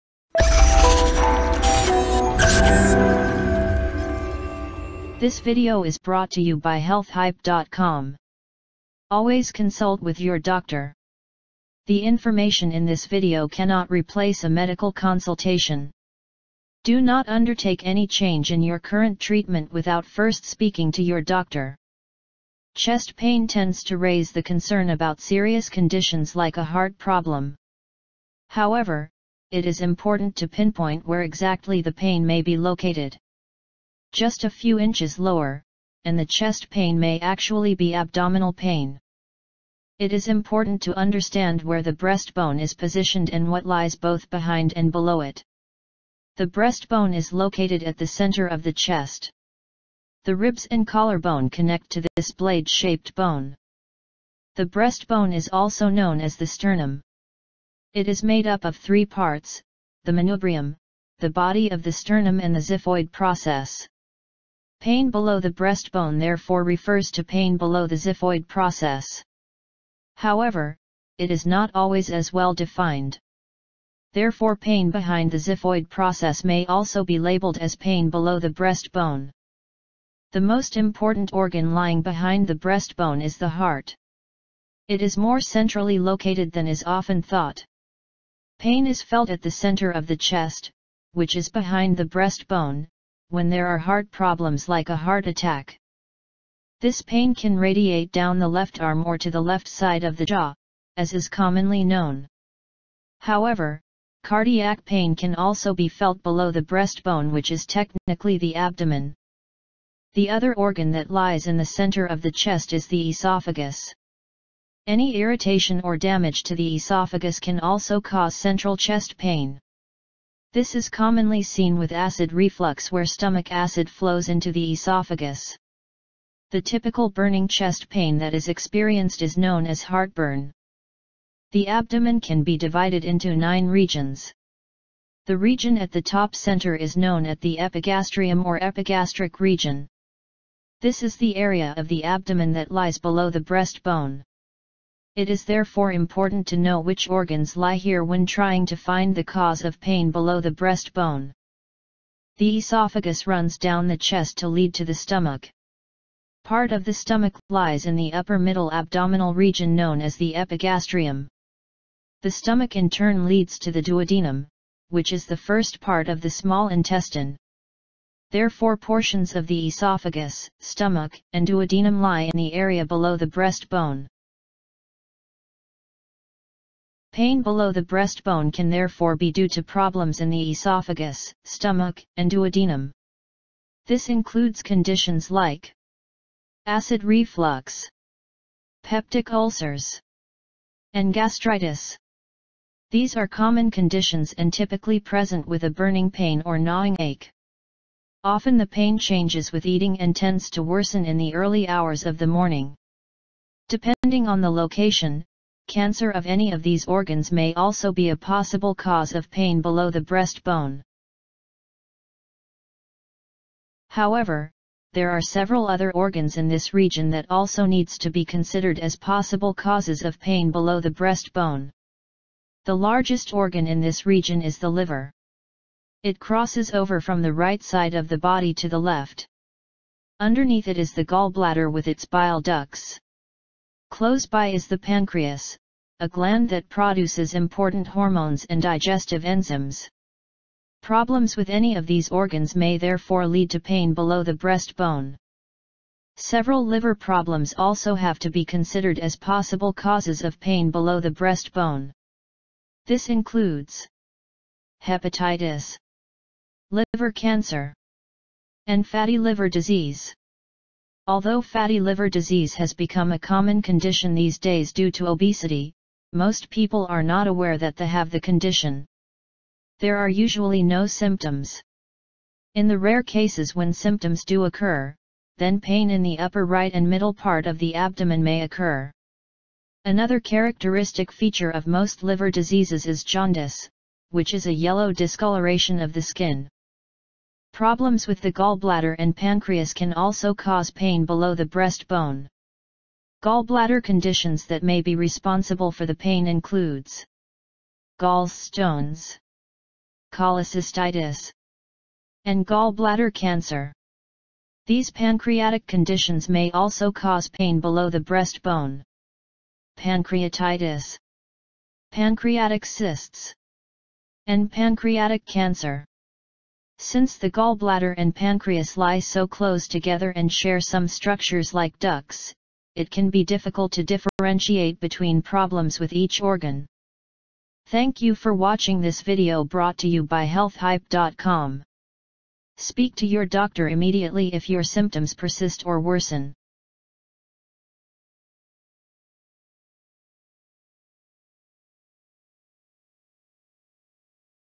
Pain Below The Breastbone (Sternum)
The causes of pain below the sternum, including heart and digestive conditions. The area below the breastbone technically lies in the upper middle abdomen.
Ask a Doctor Online Now!